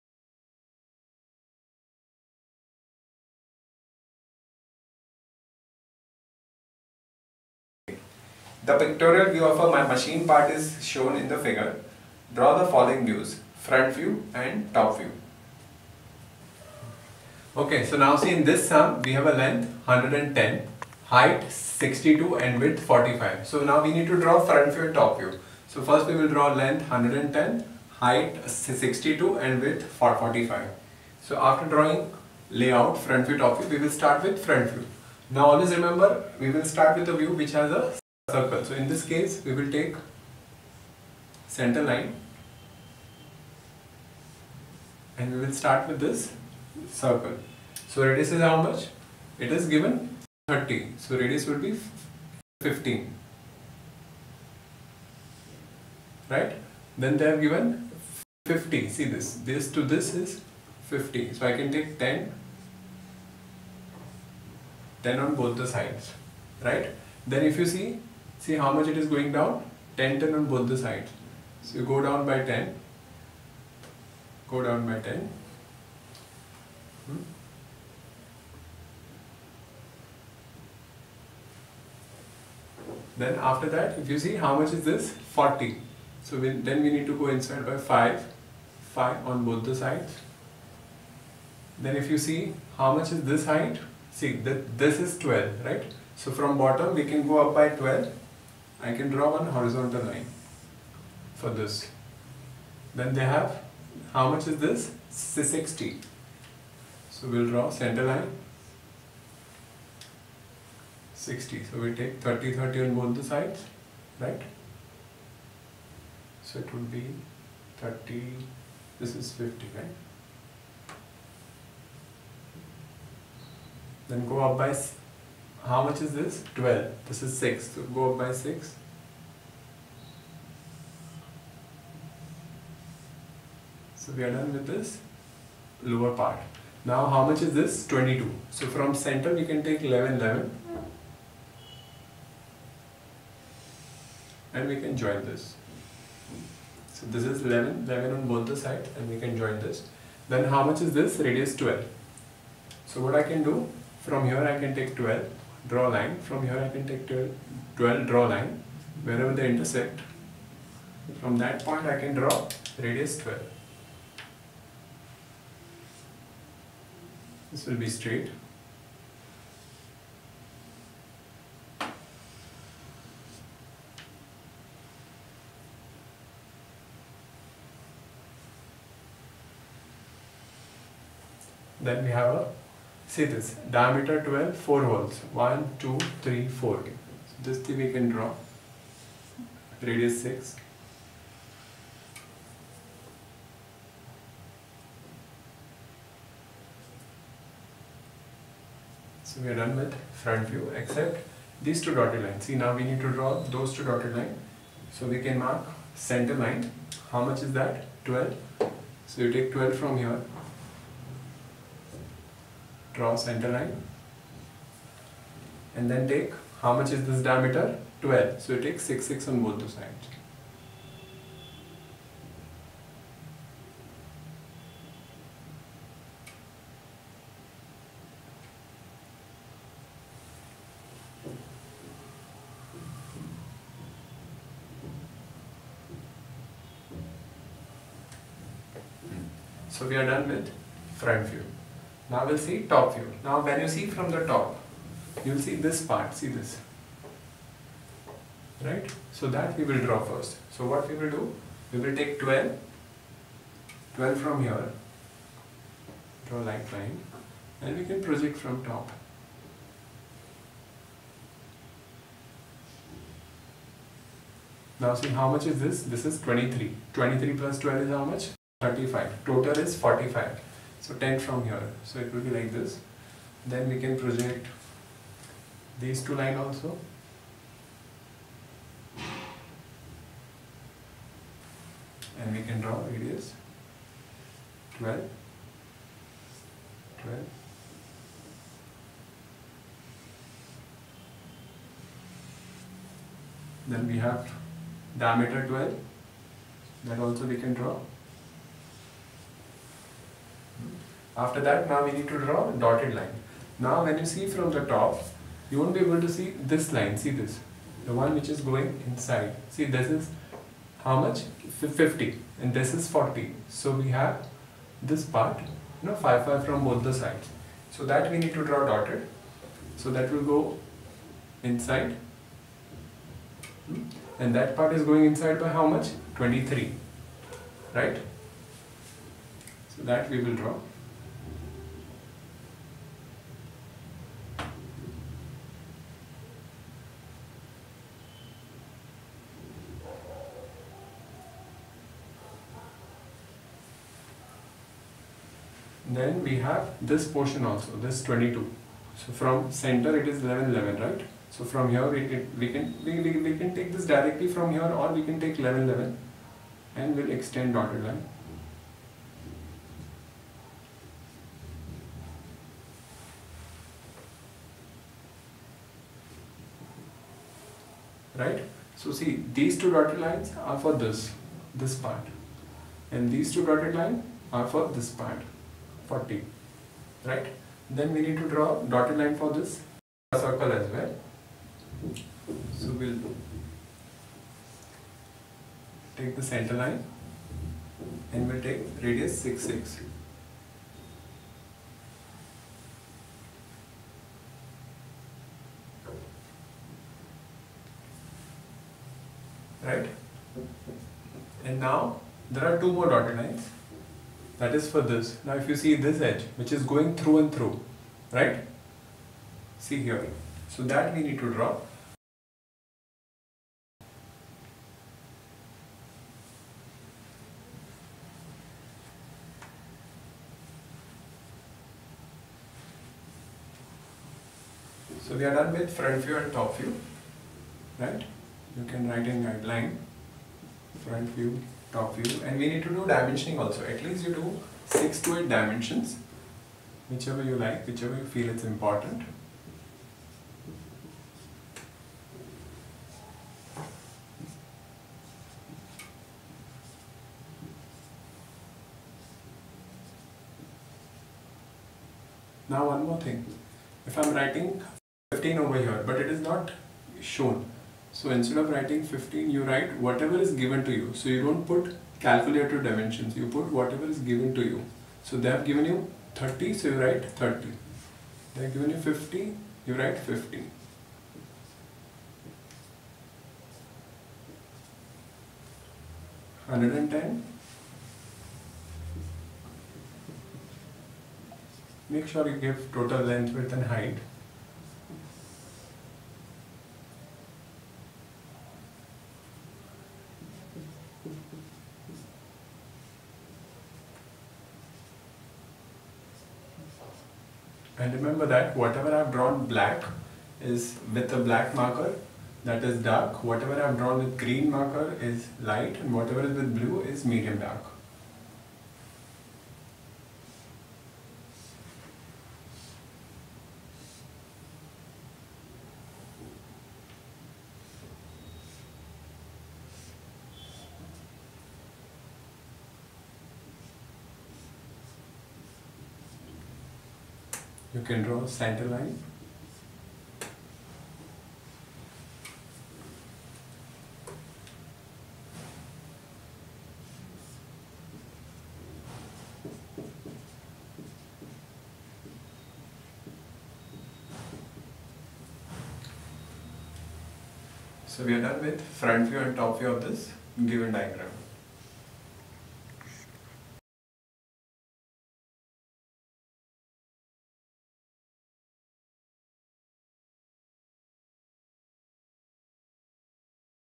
Orthographic projections - Engineering drawing 2014 Dec 1(b)
Q1 (b) The Pictorial view of a machine part is given in the figure. Draw the following views:
(i)  Front view.
(ii) Top view.